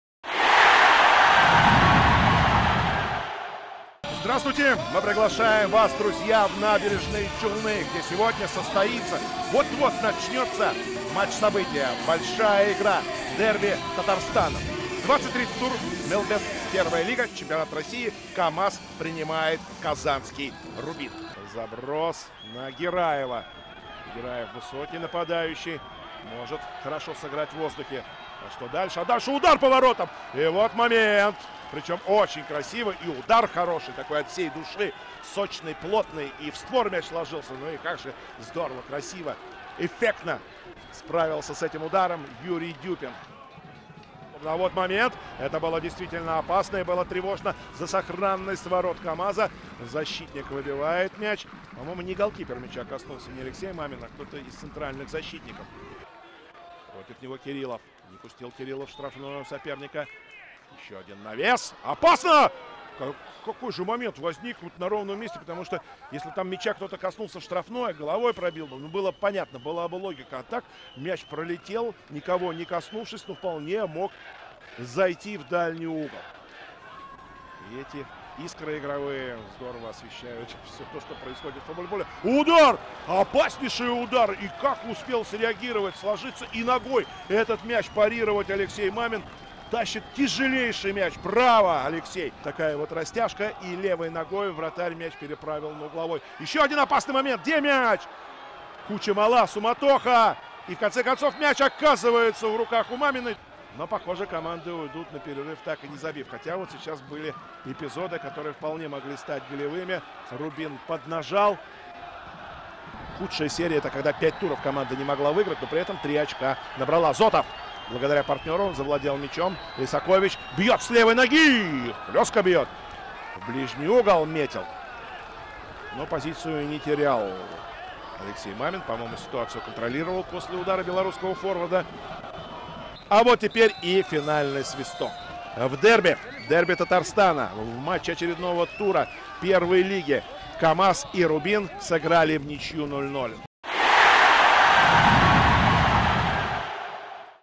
Обзор матча «КАМАЗ» — «Рубин» | 23 тур МЕЛБЕТ-Первой лиги
Лучшие моменты из игры «КАМАЗА» (Набережные Челны) и «Рубина» (Казань) 18 марта 2023.